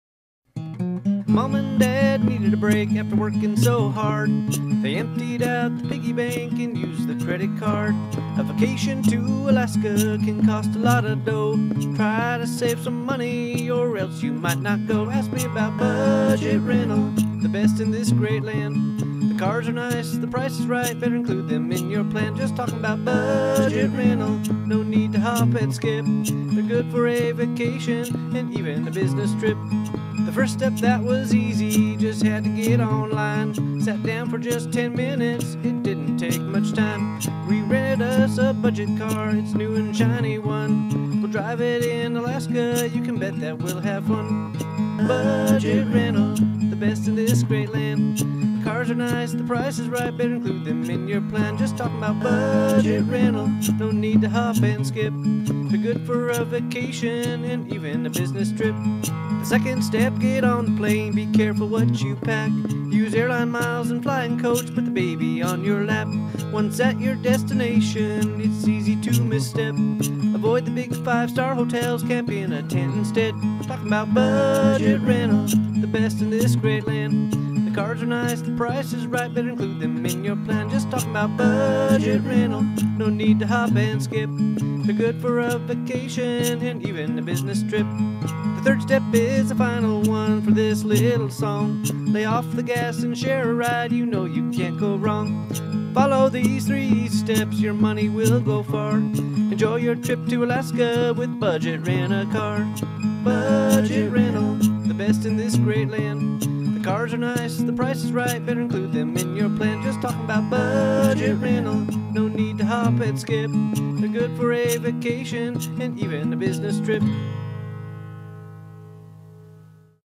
Budget Rental
A family takes an Alaskan vacation with the help of Budget Rental.